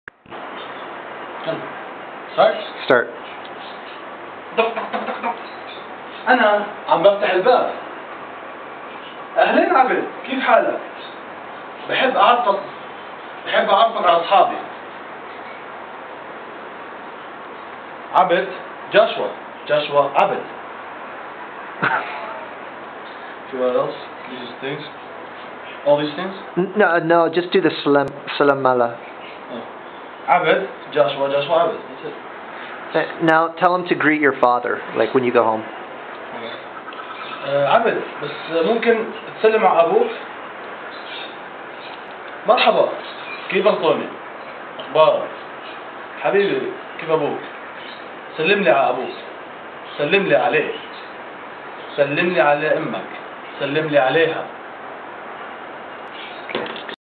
Learn Arabic: Greetings
Open door, greetings, Greet your father, hello, what's up, say hi